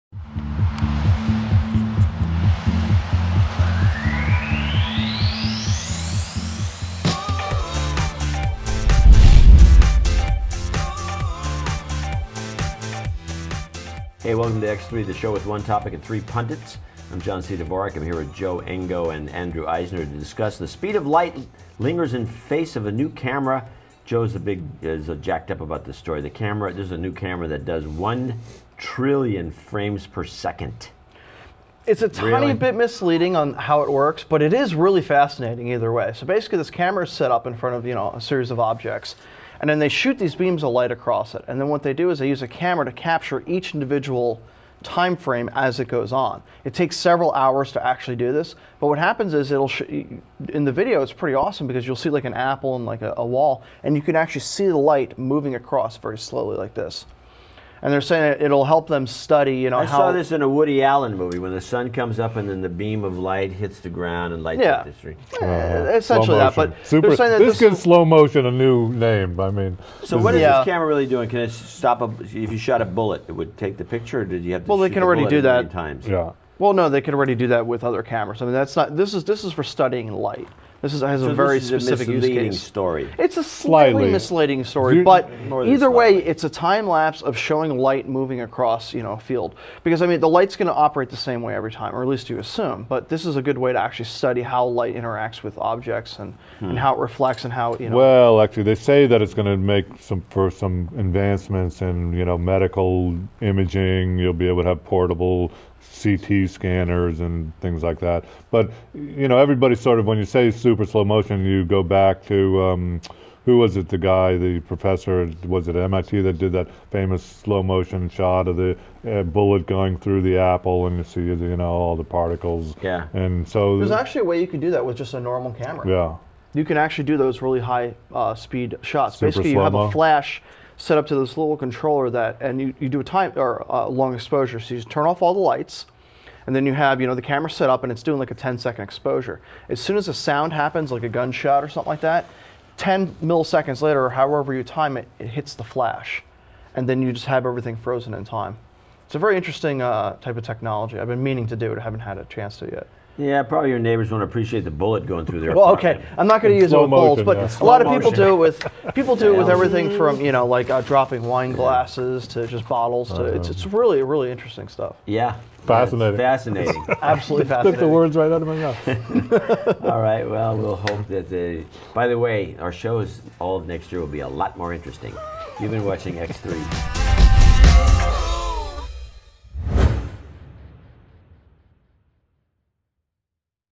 1 Trillion FPS Camera Captures Light Itself
X3 with John C. Dvorak

Guests:

Andrew Eisner, Retrevo.com

Joseph Engo, IT Specialist

Topic:

X3: 1 Trillion FPS Camera Captures Light Itself

John, Andrew, and Joe talk about a camera setup out of M.I.T. that has such a high resolution that it can capture light particles as they pass an object.